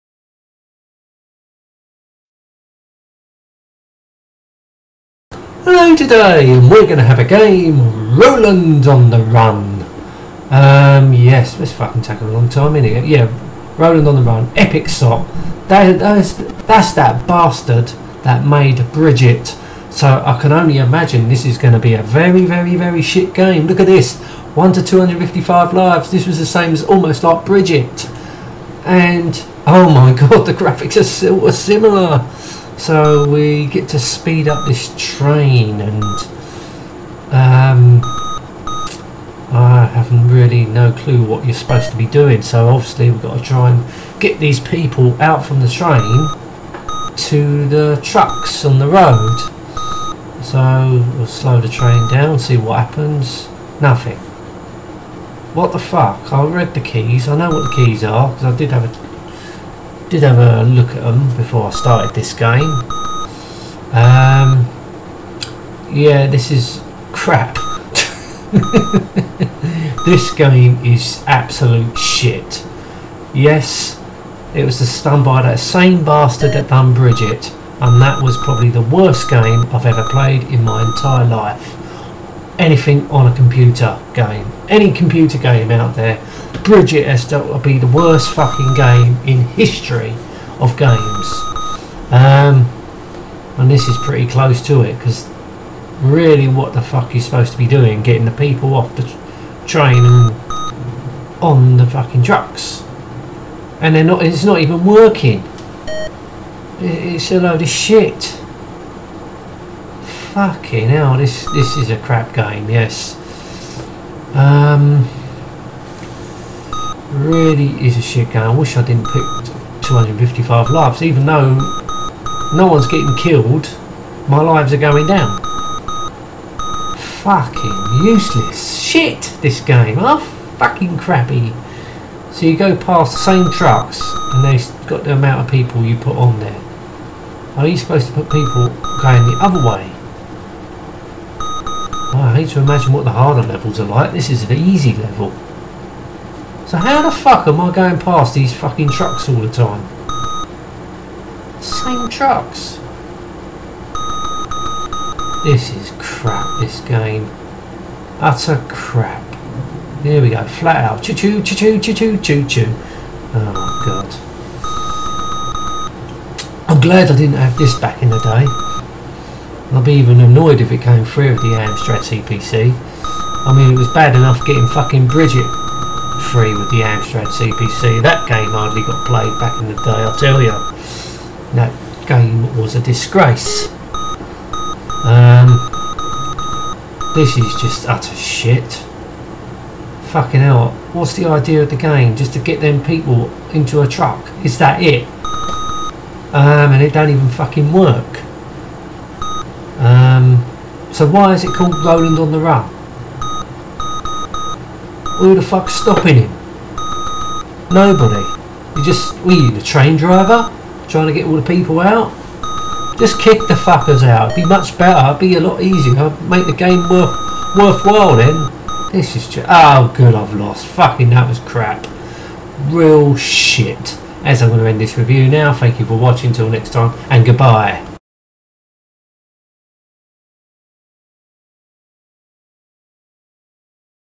Roland On The Run Review for the Amstrad CPC by John Gage
Roland In Space is a platform game that came out in 1984 by Amsoft. Warning! This video has swearing.